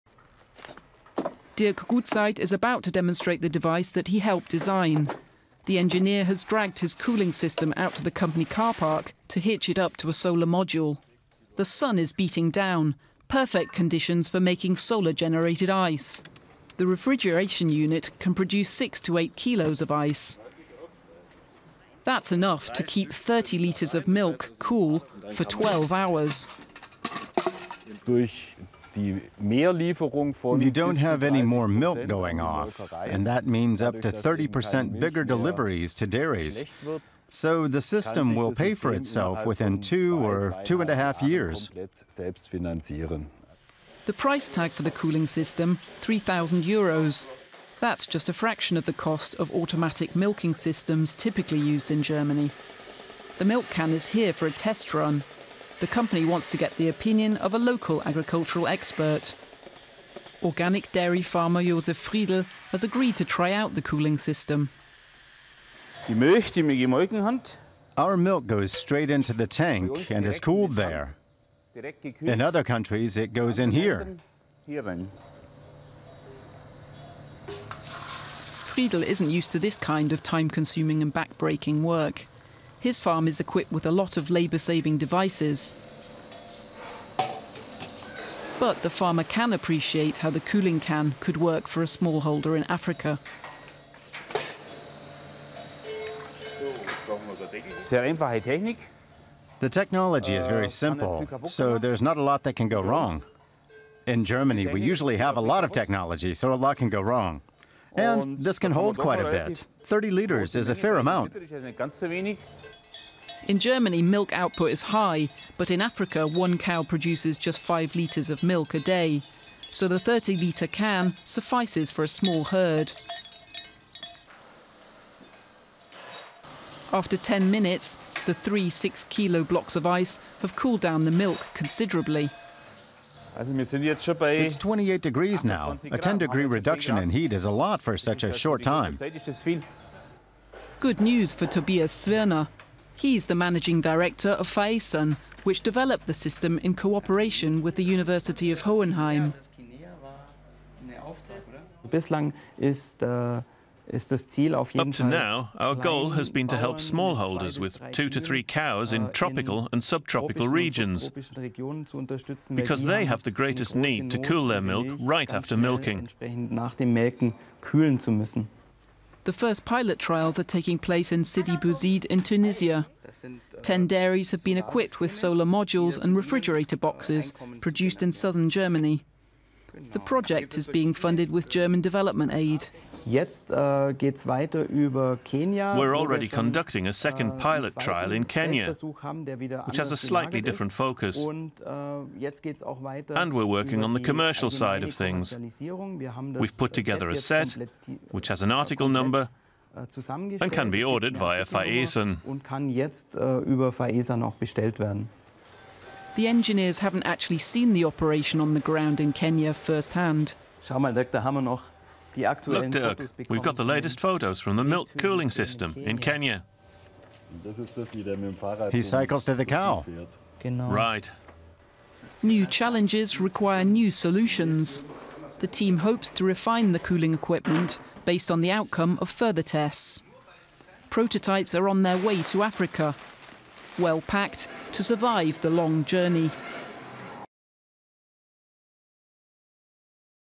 Solar-powered canisters to keep milk cool | DW English
Researchers in Germany have come up with a special solar-powered canister to keep milk cool for up to 12 hours. This could be a revolution in rural areas, where electricity is often a luxury.